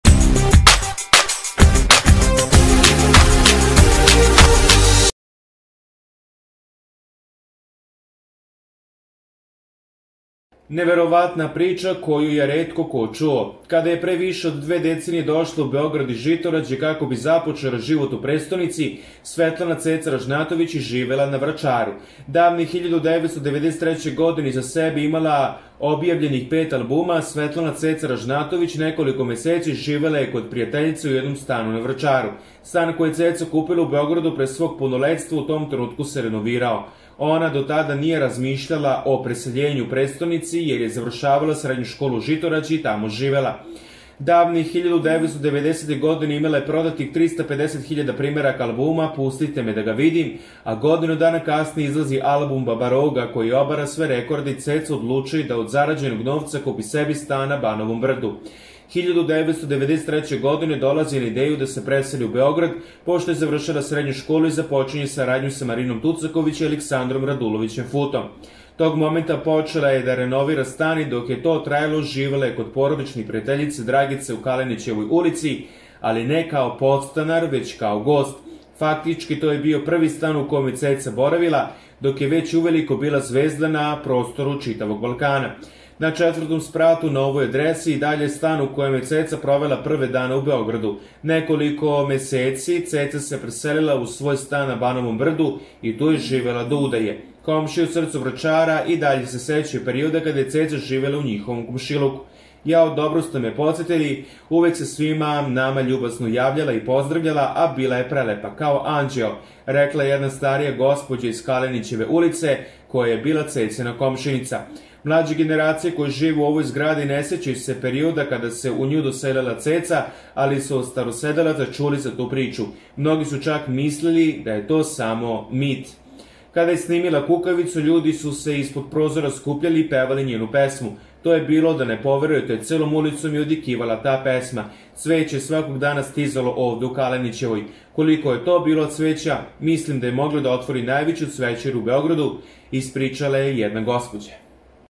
Auu! Otkrivena TAJNA koju niko nije znao! Evo gde je Ceca zivela pre udaje za Arkana
Postoji ova interesantna prica za koju niko nikada nije cuo pre